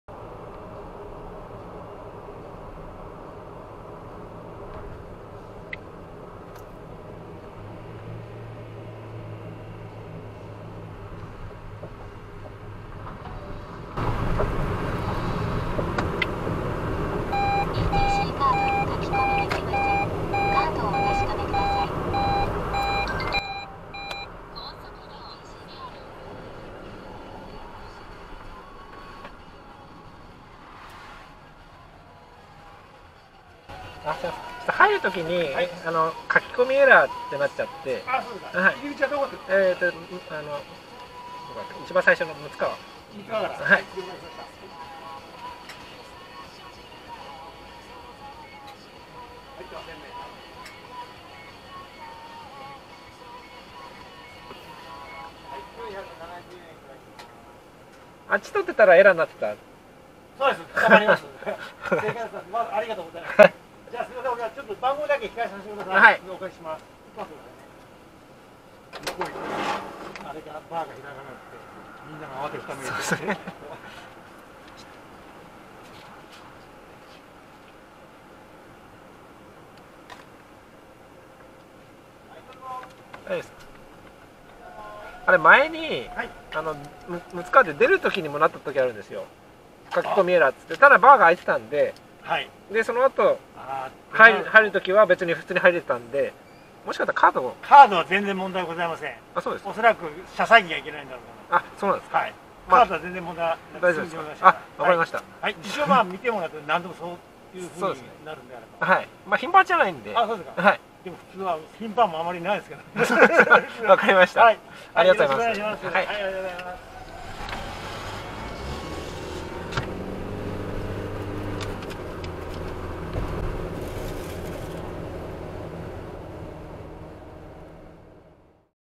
ETC書き込みエラー【対処法は？】
六ッ川料金所から入る時に書き込みエラー。開かないと怖いので有人の料金所へ。お忙しい中、色々聞かせていただきありがとうございました。ちなみに後ろからは来ていません。